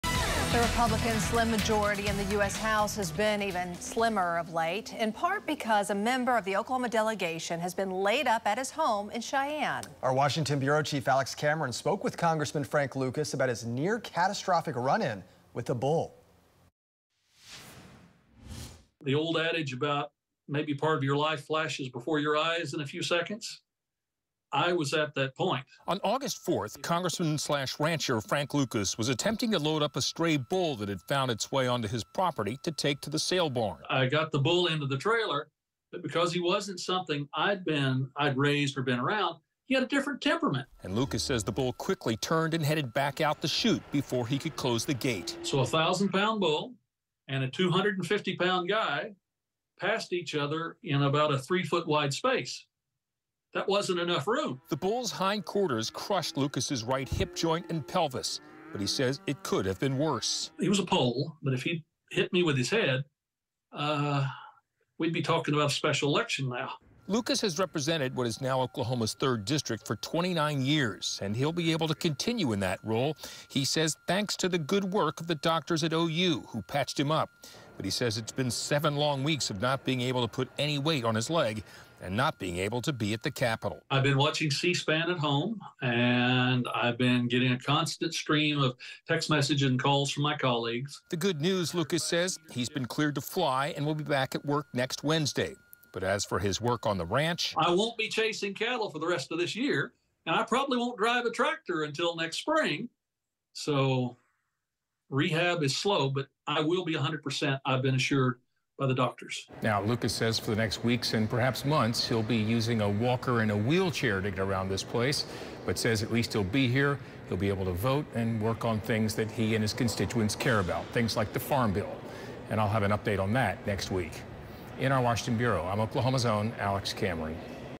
Congressman Frank Lucas Recovering From Bull Attack
Congressman Frank Lucas recovers after incident with a bull.